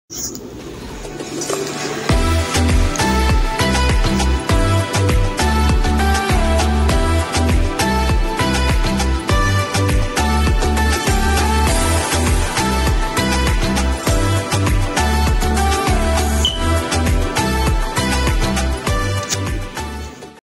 blac youngsta shoot his new video at Young dolph Grave site
blac youngsta young dolph#yo gotti#cmg#paper route